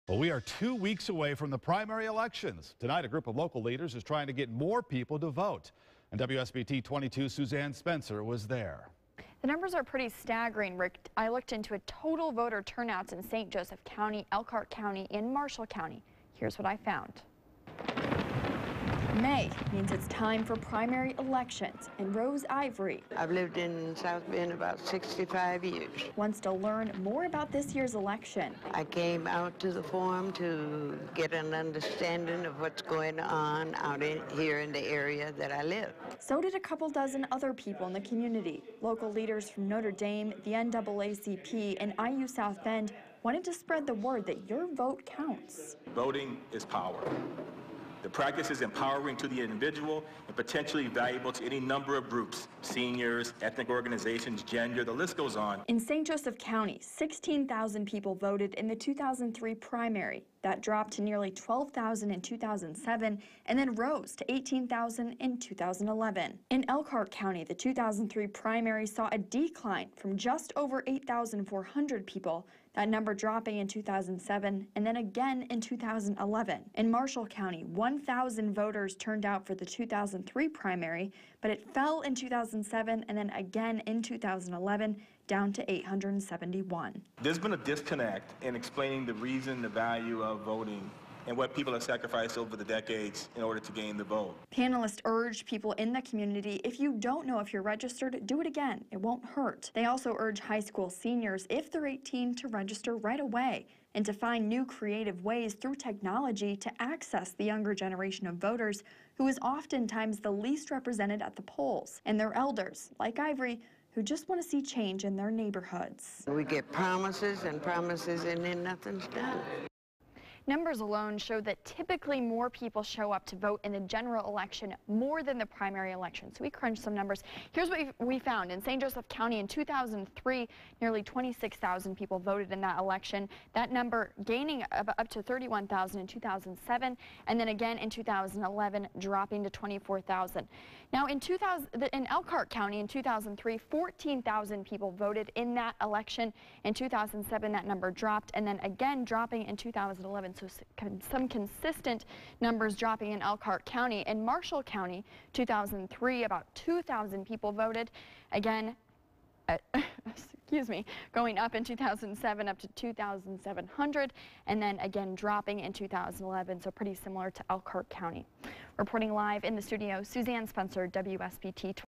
Community leaders urge voting
We are two weeks away from the primary elections. Tuesday-- a group of local leaders is trying to get more people to vote. _________________